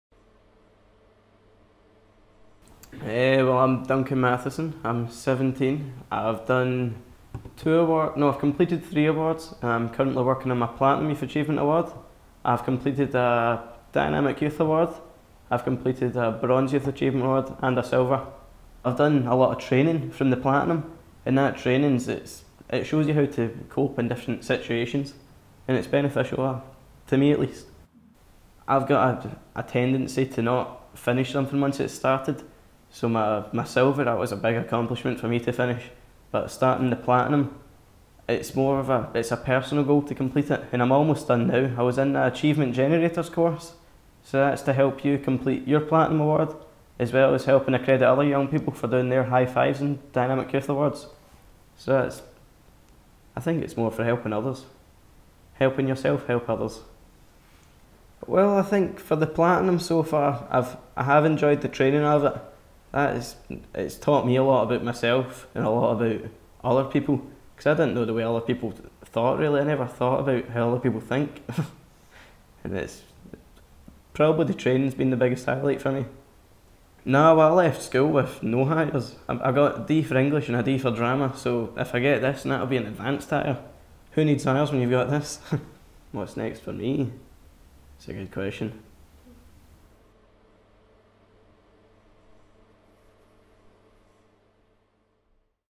Duncan's story - a Youth Achievement Awards case study
Duncan, 17, has a Dynamic Youth Award (DYA), a Bronze YAA and a Silver YAA, and is working towards his Platinum YAA. He did his DYA at school and was introduced to YAA at a Universal Connections, community-based youth learning centre. Duncan has developed experience and skills, including volunteering, leadership, cooking and cash handling. His YAA gave him a chance to get qualifications he would not have achieved at school.